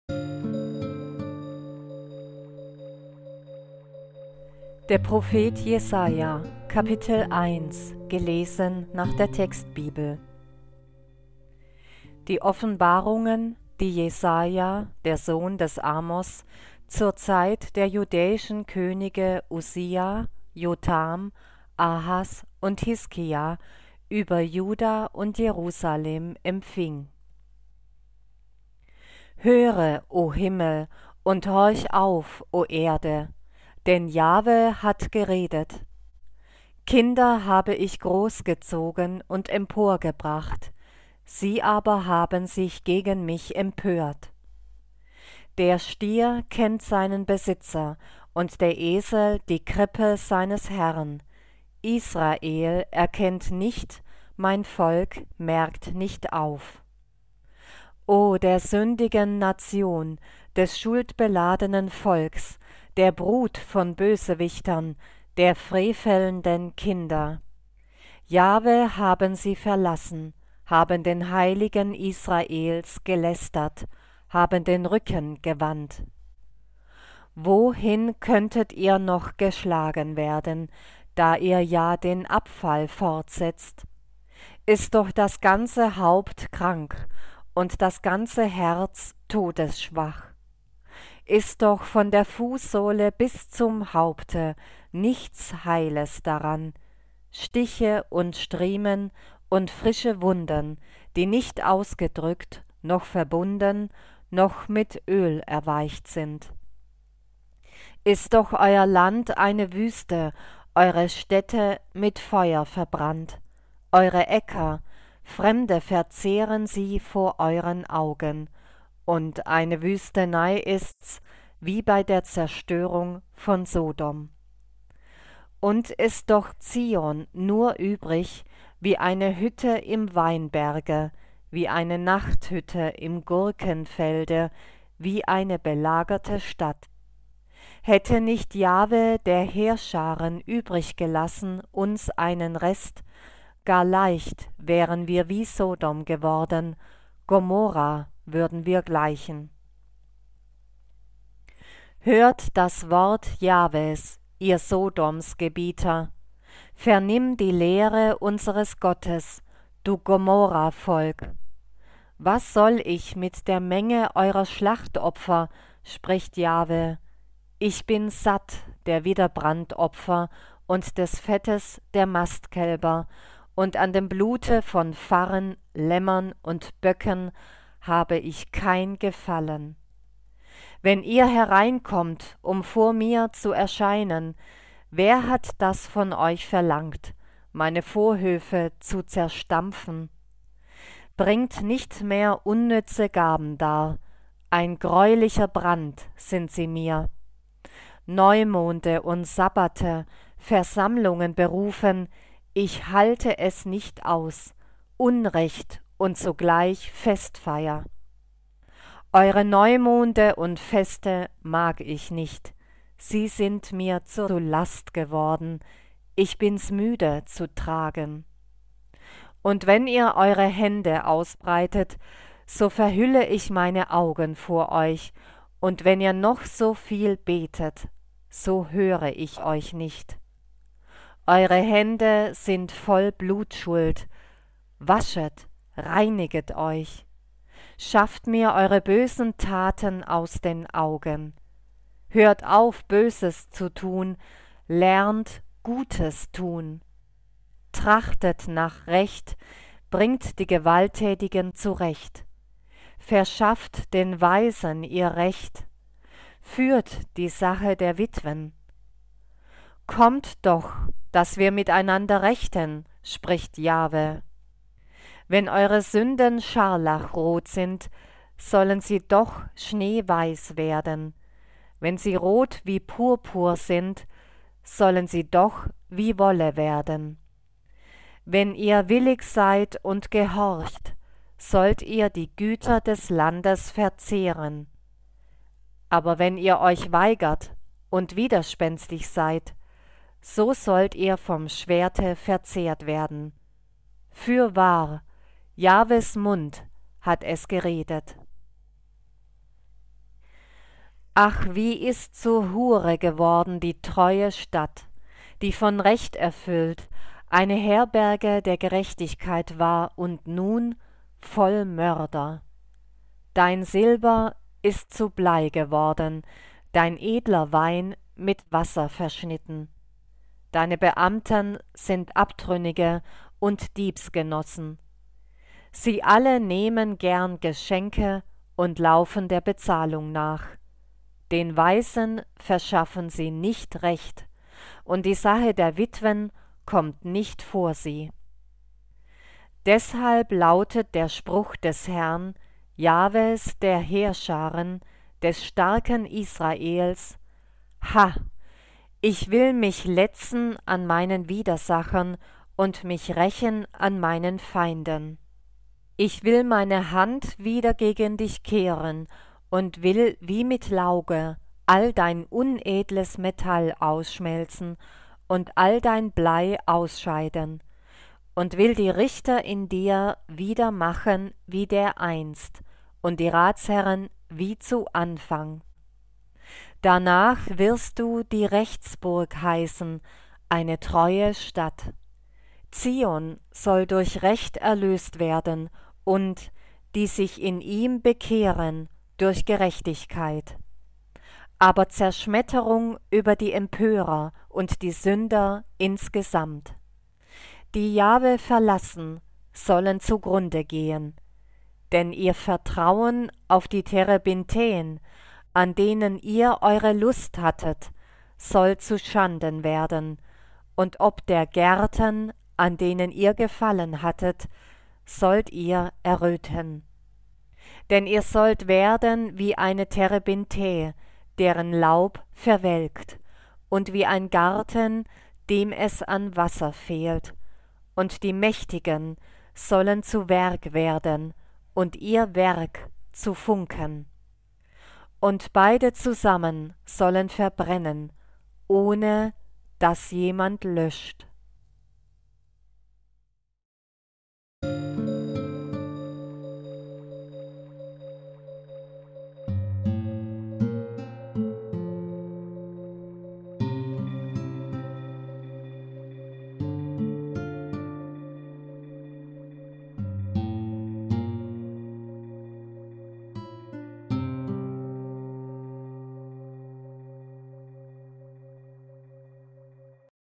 Jesaja 1 - Klage und Anklage Jahwes wider Gesetzlosigkeit und Heidentum seines Volkes Israel.
Der Prophet Jesaja.

Kapitel 1.

1 Die Offenbarungen, die Jesaja, der Sohn des Amoz, zur Zeit der judäischen Könige Usia, Jotham, Ahas und Hiskia über Juda und Jerusalem empfing.

2 Höre, o Himmel, und horch auf, o Erde! Denn Jahwe hat geredet:
Kinder habe ich großgezogen und emporgebracht; sie aber haben sich gegen mich empört.
3 Der Stier kennt seinen Besitzer und der Esel die Krippe seines Herrn - Israel erkennt nicht, mein Volk merkt nicht auf!
4 O der sündigen Nation, des schuldbeladenen Volks,
der Brut von Bösewichtern, der frevelnden Kinder!
Jahwe haben sie verlassen, haben den Heiligen Israels gelästert,
haben den Rücken gewandt.
5 Wohin könntet ihr noch geschlagen werden, da ihr ja den Abfall fortsetzt?
Ist doch das ganze Haupt krank und das ganze Herz todesschwach!
6 Ist doch von der Fußsohle bis zum Haupte nichts Heiles daran:
Stiche und Striemen und frische Wunden,
die nicht ausgedrückt, noch verbunden, noch mit Öl erweicht sind!
7 Ist doch euer Land eine Wüste, eure Städte mit Feuer verbrannt.
Eure Äcker - Fremde verzehren sie vor euren Augen -
und eine Wüstenei ist's, wie bei der Zerstörung von Sodom!
8 Und ist doch Zion nur übrig, wie eine Hütte im Weinberge,
wie eine Nachthütte im Gurkenfelde, wie eine belagerte Stadt!
9 Hätte nicht Jahwe der Heerscharen übrig gelassen uns einen Rest,
gar leicht wären wir wie Sodom geworden, Gomorra würden wir gleichen!

10 Hört das Wort Jahwes, ihr Sodomsgebieter!
Vernimm die Lehre unseres Gottes, du Gomorravolk!
11 Was soll ich mit der Menge eurer Schlachtopfer? - spricht Jahwe.
Ich bin satt der Widderbrandopfer und des Fettes der Mastkälber,
und an dem Blute von Farren, Lämmern und Böcken habe ich kein Gefallen.
12 Wenn ihr hereinkommt, um vor mir zu erscheinen -
wer hat das von euch verlangt, meine Vorhöfe zu zerstampfen?
13 Bringt nicht mehr unnütze Gaben dar - ein greulicher Brand sind sie mir!
Neumonde und Sabbate, Versammlungen berufen - ich halte es nicht aus: Unrecht und zugleich Festfeier!
14 Eure Neumonde und Feste mag ich nicht,
sie sind mir zur Last geworden, ich bin's müde zu tragen.
15 Und wenn ihr eure Hände ausbreitet, so verhülle ich meine Augen vor euch,
und wenn ihr noch so viel betet, so höre ich euch nicht.
Eure Hände sind voll Blutschuld!  16 Wascht, reinigt euch!
Schafft mir eure bösen Taten aus den Augen!
Hört auf, Böses zu tun!  17 Lernt Gutes tun!
Trachtet nach Recht! Bringt die Gewalttätigen zurecht!
Verschafft den Waisen ihr Recht! Führt die Sache der Witwen!
18 Kommt doch, dass wir miteinander rechten! - spricht Jahwe.
Wenn eure Sünden scharlachrot sind, sollen sie doch schneeweiß werden;
wenn sie rot wie Purpur sind, sollen sie doch wie Wolle werden.
19 Wenn ihr willig seid und gehorcht, sollt ihr die Güter des Landes verzehren.
20 Aber wenn ihr euch weigert und widerspenstig seid, so sollt ihr vom Schwerte verzehrt werden!
Fürwahr, Jahwes Mund hat es geredet!

21 Ach wie ist zur Hure geworden die treue Stadt,
die von Recht erfüllt, eine Herberge der Gerechtigkeit war und nun - voll Mörder!
22 Dein Silber ist zu Blei geworden, dein edler Wein mit Wasser verschnitten.
23 Deine Beamten sind Abtrünnige und Diebsgenossen.
Sie alle nehmen gern Geschenke und laufen der Bezahlung nach;
den Waisen verschaffen sie nicht Recht, und die Sache der Witwen kommt nicht vor sie.
24 Deshalb lautet der Spruch des Herrn, Jahwes der Heerscharen, des Starken Israels:
Ha! Ich will mich letzen an meinen Widersachern und mich rächen an meinen Feinden!
25 Ich will meine Hand wieder gegen dich kehren
und will wie mit Lauge all' dein unedles Metall ausschmelzen
und all' dein Blei ausscheiden
26 und will die Richter in dir wieder machen wie dereinst und die Ratsherren wie zu Anfang.
Danach wirst du die Rechtsburg heißen, eine treue Stadt.
27 Zion soll durch Recht erlöst werden und, die sich in ihm bekehren, durch Gerechtigkeit;
28 aber Zerschmetterung über die Empörer und die Sünder insgesamt! Die Jahwe verlassen, sollen zugrunde gehen!
29 Denn ihr Vertrauen auf die Terebinthen, an denen ihr eure Lust hattet, soll zu Schanden werden,
und ob der Gärten, an denen ihr Gefallen hattet, sollt ihr erröten.
30 Denn ihr sollt werden wie eine Terebinthe, deren Laub verwelkt,
und wie ein Garten, dem es an Wasser fehlt,
31 und die Mächtigen sollen zu Werg werden und ihr Werk zu Funken,
und beide zusammen sollen verbrennen, ohne dass jemand löscht.